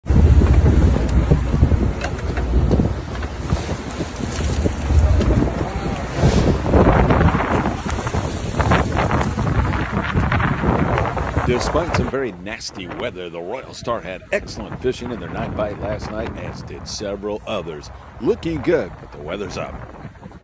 Southern California anglers battle windy, rough seas at night to catch big bluefin tuna at night
Excellent nighttime fishing for big bluefin tuna near San Clemente Island at night for Southern California Anglers on board the Royal Star.